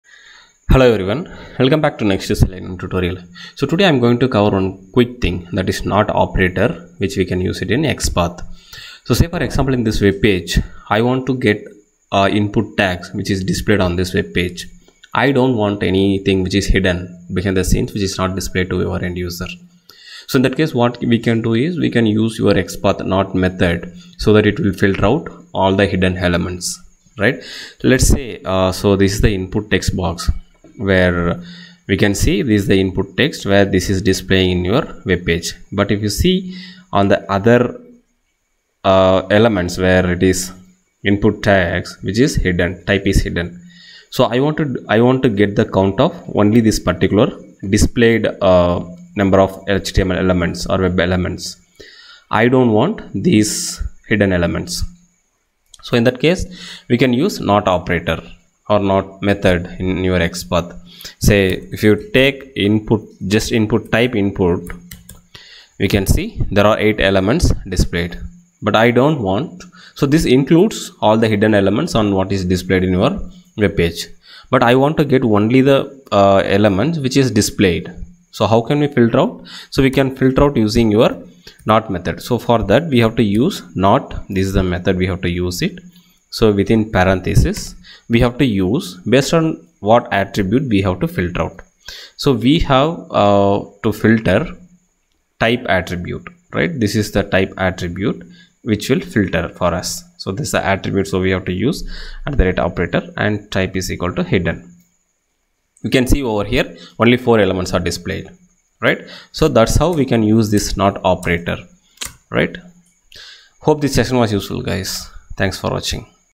How To Use not Operator in XPath Selenium WebDriver || Java
not operator or function or method is one of the xpath logical operator and is used in creating dynamic xpath for unstable web application. In this tutorial let's learn detailed view of the not Boolean or logical operator. not operator can be used in multiple ways.
Syntax
//tagName[not(@attribute=‘value’]
//tagName[@attribute=‘value’ and not(text()=‘value’)]